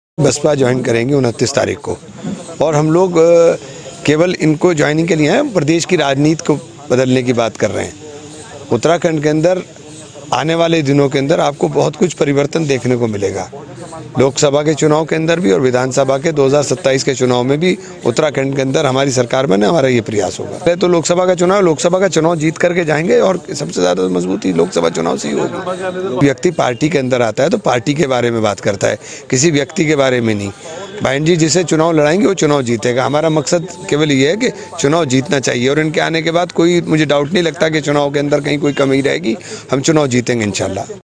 बसपा नेता इमरान मसूद का बड़ा बयान- जल्द सोनिया शर्मा बसपा में होगी शामिल || BSP | Mayawati
हमसे जुड़े -